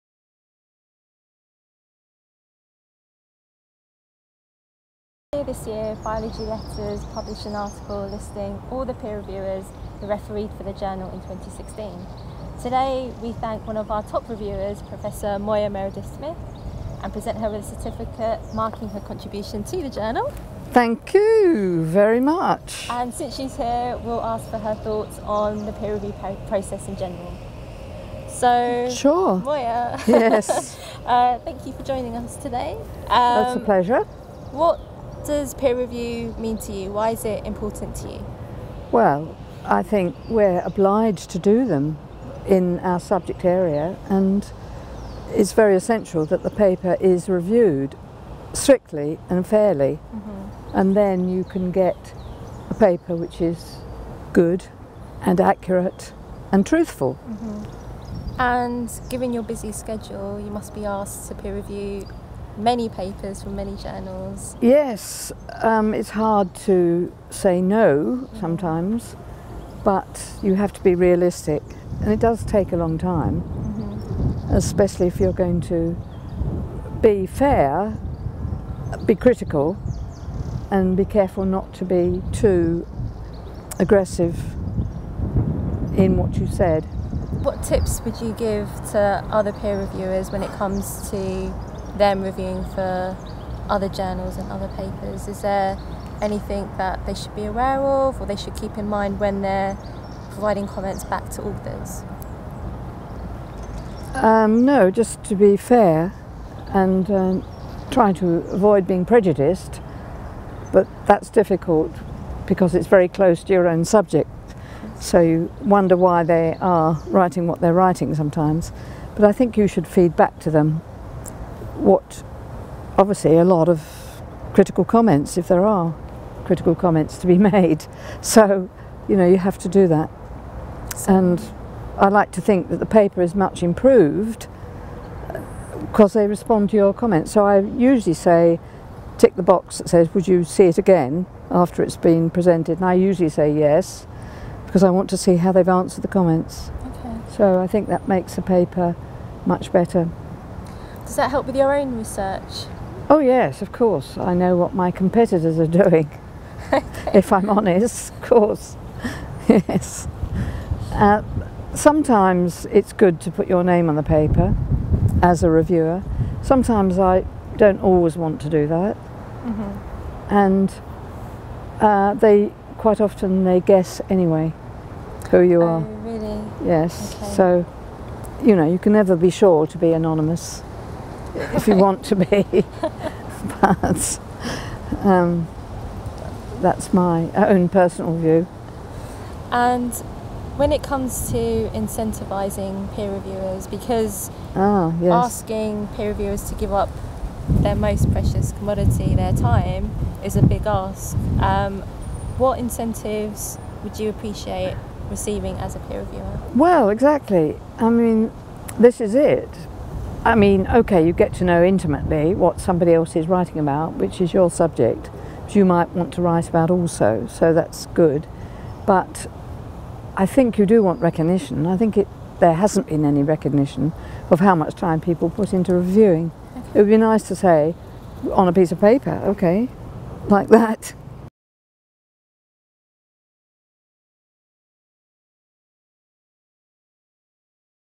Meet a Biology Letters top reviewer
Biology Letters meets one of its most committed reviewers, Moya Meredith Smith of King’s College London, UK. She discusses reviewing, motivation and how she would like to be rewarded.

Biology Letters published its top reviewer list as a standalone article, you can see the full list